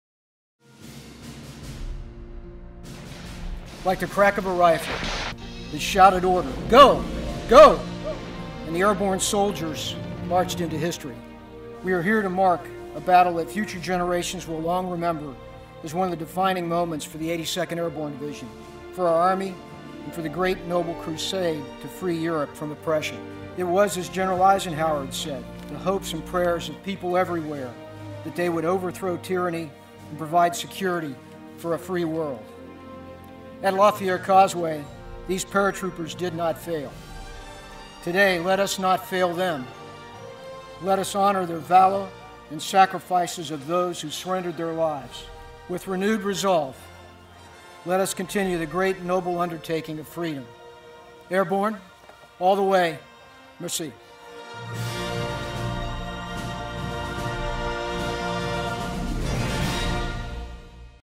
Iron Mike
General Curtis Scaparrotti, Commander of the  United States European Command spoke during 74th anniversary commemoration ceremonies at the Iron Mike monument at the La Fière causeway near Sainte-Mere-Eglise Normandy, France. Scaparrotti praised the valor and sacrifice of the soldiers who put greater purpose and needs above their own.  This year marks the 74th anniversary of Operation Overlord, the Allied invasion of Normandy on June 6, 1944 - most commonly known as D-Day. an epic multinational amphibious and airborne operation, D-Day forged partnerships and reinforced transatlantic bonds that remain strong today. Overall, U.S. service members from 20 units in Europe and the U.S. will participate in events and ceremonies May 30 - June 7, 2018, in almost 40 locations throughout the Normandy region of France as part of Joint Task Force Normandy 74.